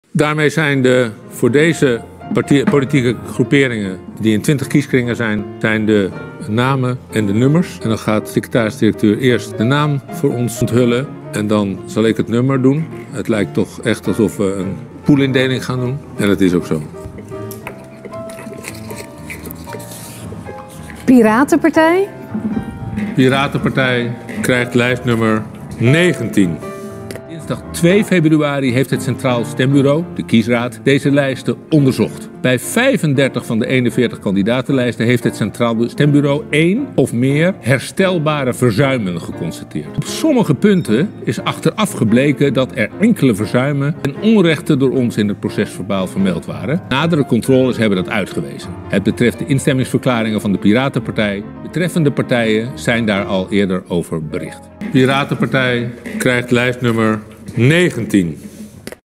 Piratenpartij Lijst 19 TK2021
De Piratenpartij zal in alle 20 kieskringen op het stemformulier staan.
Er moest geloot worden voor een plek na de zittende partijen.
De Piratenpartij zal op lijst 19 staan.